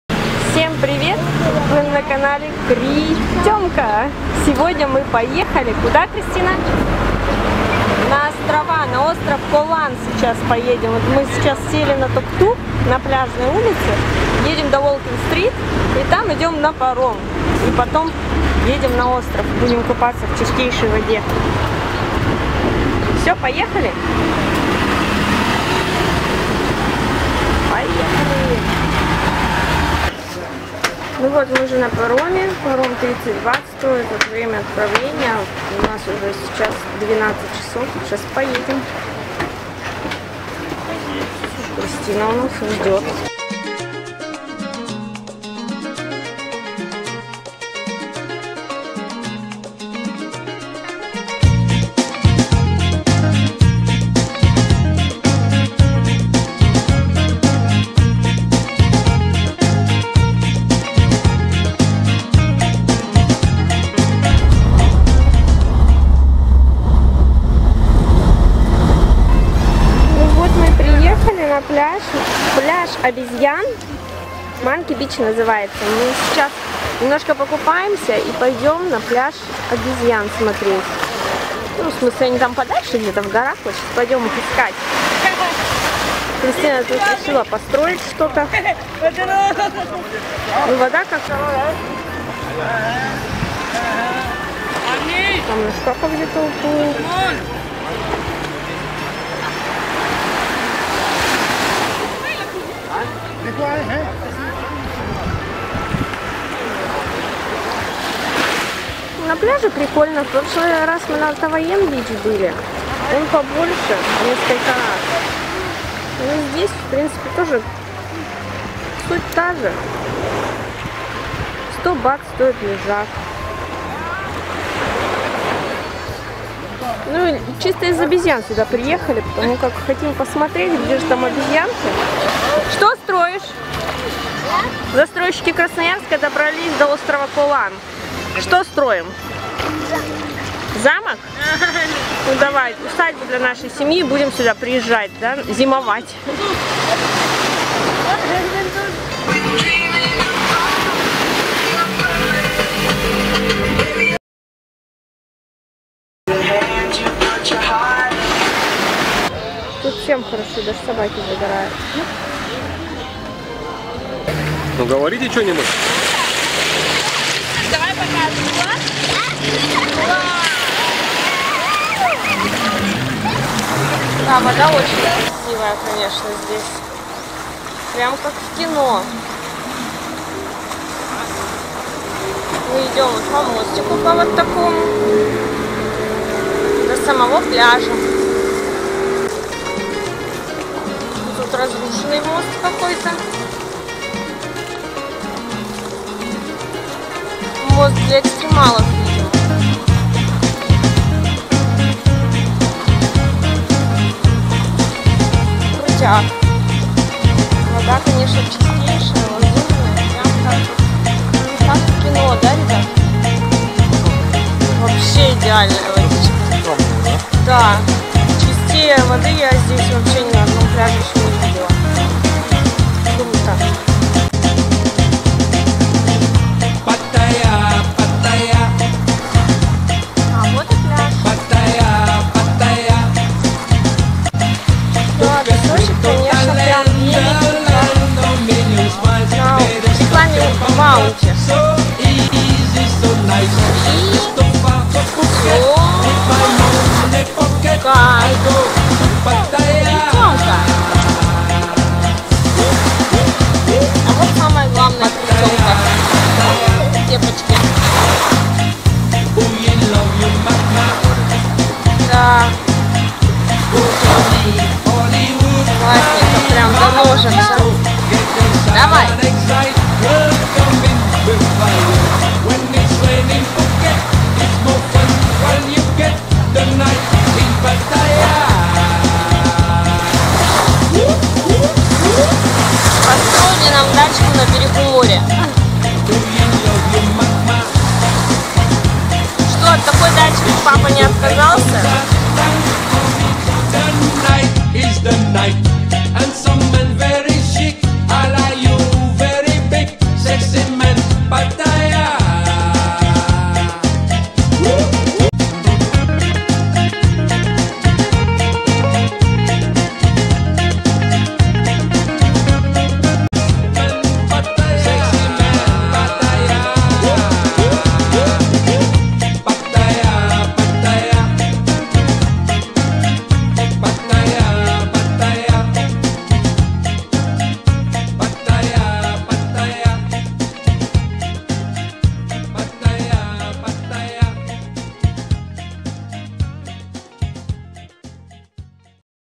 пляжи острова КОЛАН невероятные приключения русских в Тайланде едем на бомбические пляжи купаться
Мы посетили несколько пляжей на близлежащем к Паттайе острове Колан. А точнее мы посетили 3 пляжа из 6: Манки бич (пляж обезьян), Тааян бия, Тьен бич. На Тааян бич спрятан наш клад!!! На пляжах очень круто! Лазурная вода, белый чистый песочек и расслабленная обстановка!!! Мы и накупались, и назагорались и даже нам построили дачу у моря!!! Сморите как это было!